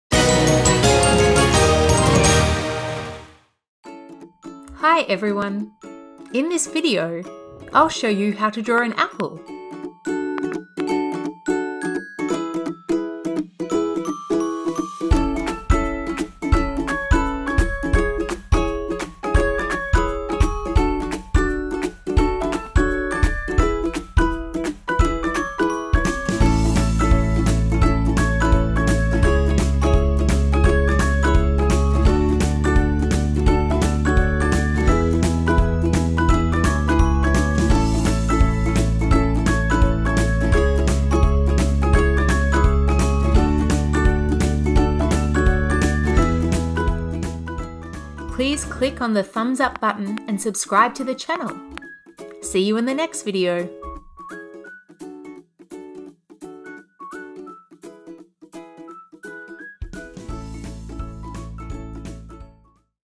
How To Draw An Apple Step By Step | Apple Drawing EASY | Super Easy Drawing Tutorials
Hi Everyone,
In today's drawing tutorial I show you how to draw an apple! If you want to make your own apple drawing easily do not hesitate to watch this video!